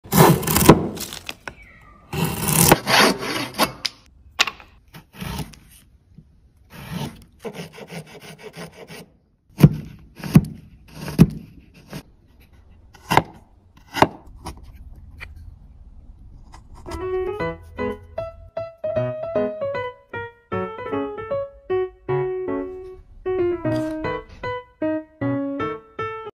Carving A Small Mushroom｜Wood cutting ASMR｜Kelvin's Wood
ASMR Woodworking Woodcarving We emphasize the compliance of the video content and there is absolutely no bad guidance or dangerous behavior.